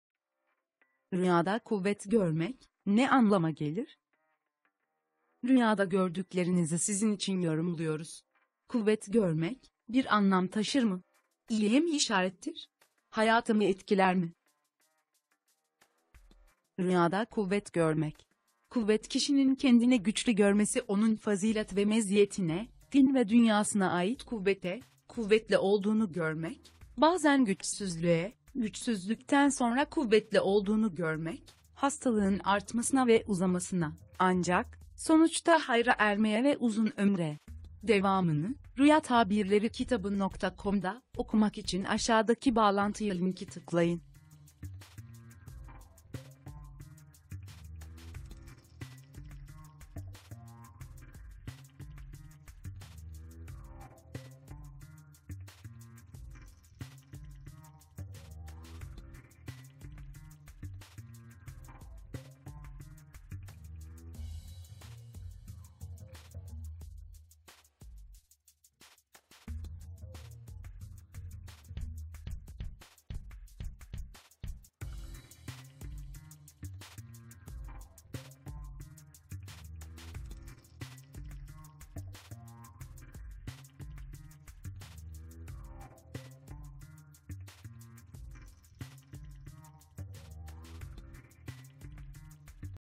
Rüyada Kuvvet Görmek Ne Anlama Gelir, Ne demektir?
Rüyada Kuvvet Görmenin Anlamı Nedir? Öğrenmek için
Kuvvet Kişinin kendini güçlü görmesi onun fazilet ve meziyetine, din ve dünyasına ait kuvvete, Kuvvetli olduğunu görmek bazen güçsüzlüğe, Güçsüzlükten sonra kuvvetli olduğunu görmek hastalığın artmasına ve uzamasına; ancak, sonuçta hayra ermeye ve uzun ömre

Rüyada Kuyruk Görmek (bir sonraki rüya).

Rüyada Kutu Görmek (bir önceki rüya).